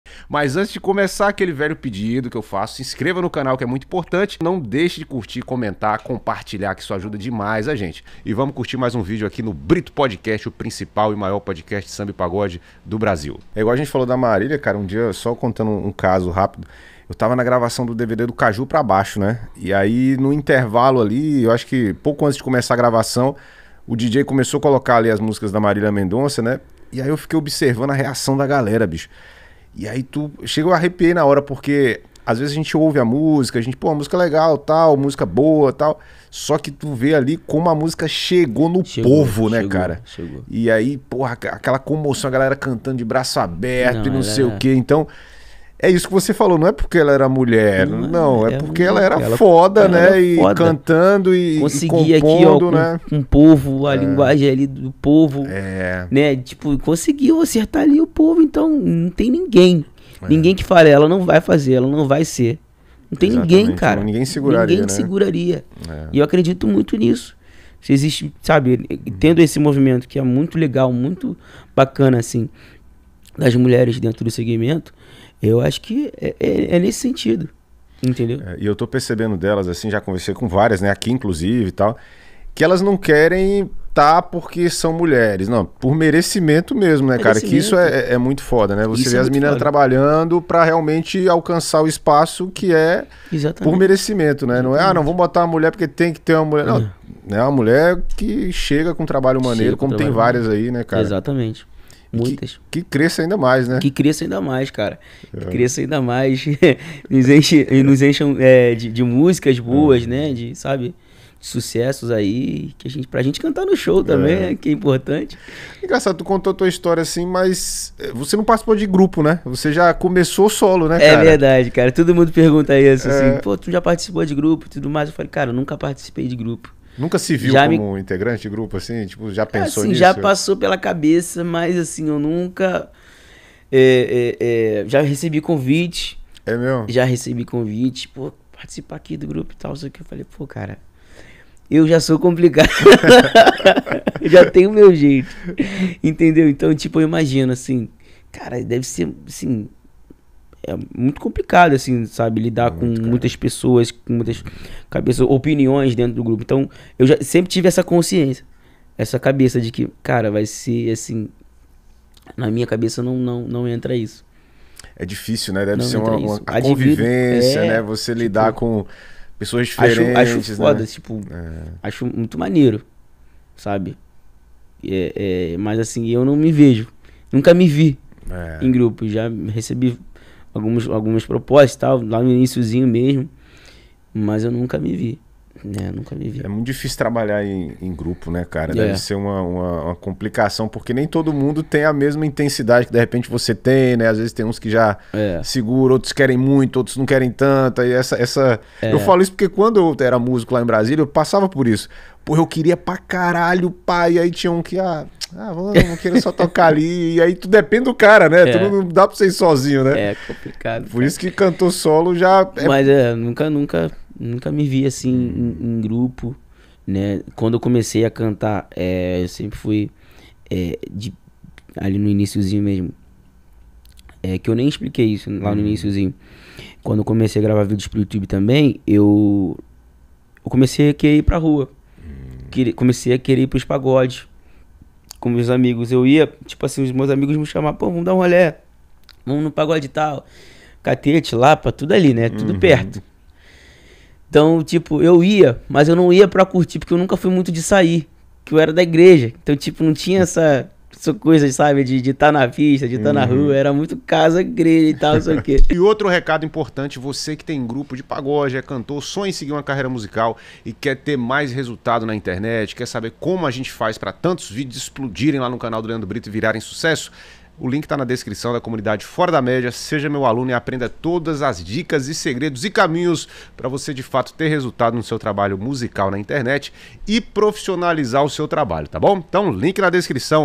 GUGA NANDES SOBRE SER UM CANTOR SOLO | Brito Podcast
Confira na integra esse super papo com Guga Nandes

ACESSE JÁ e aprenda todos os caminhos para o seu trabalho musical crescer no mundo digital.